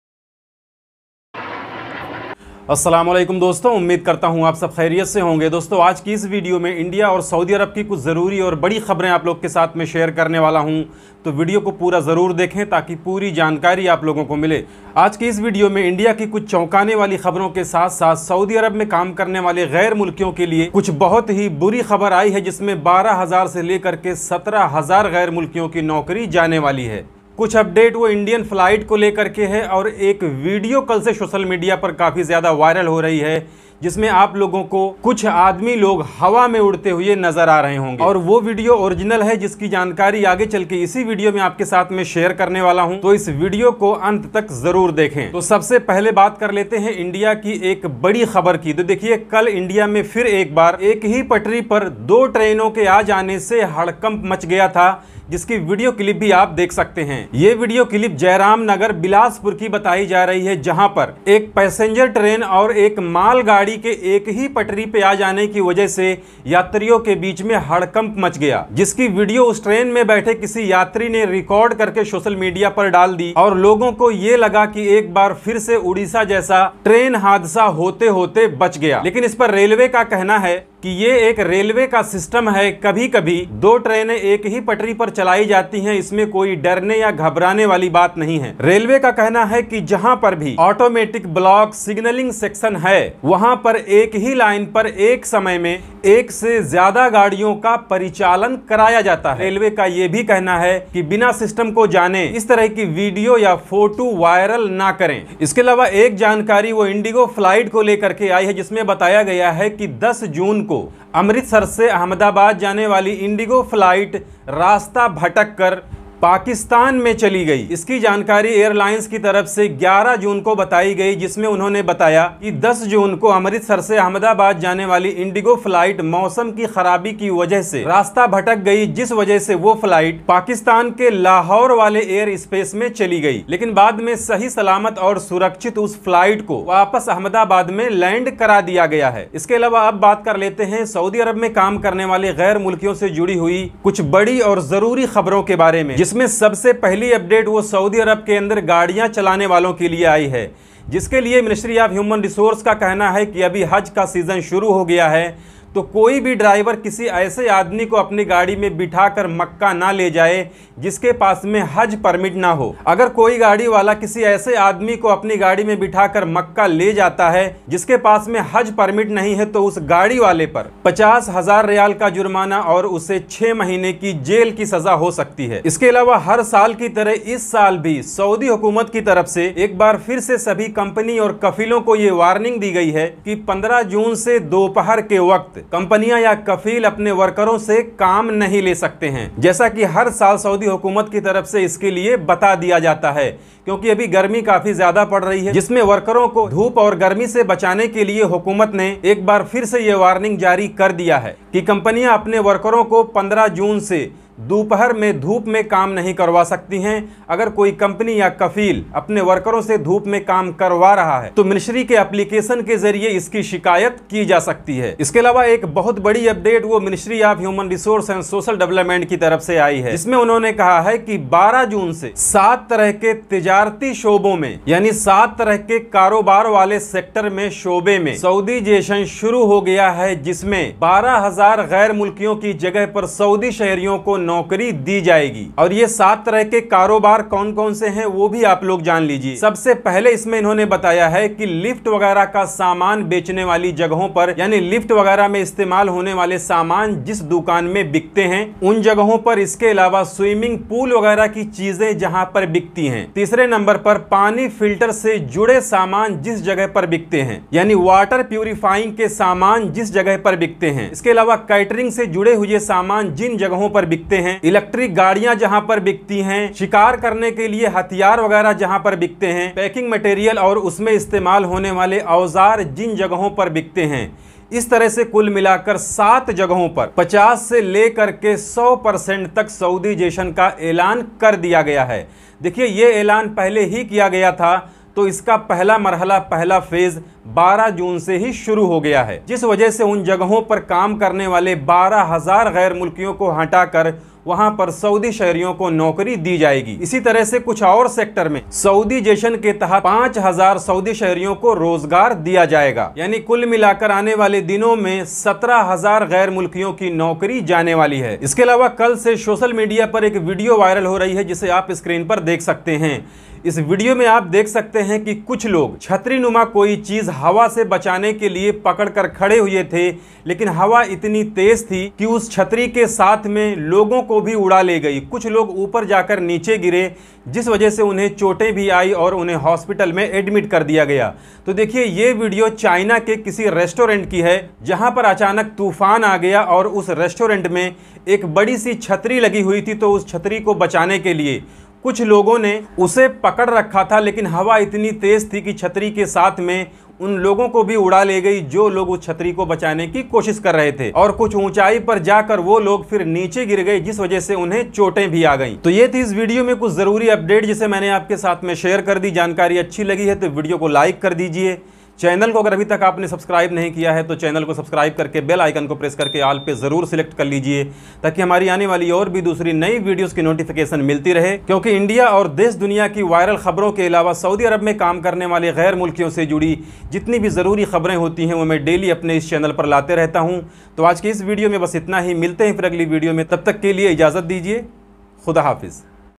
सऊदी मे हड़कंप मच गया हज़ारो इन्डियन वर्कर लोग नौकरी से बाहर #theziavlog
Latest Update for expatriates in saudi Arabia | Latest Indian viral news | Saudization in seven business outlets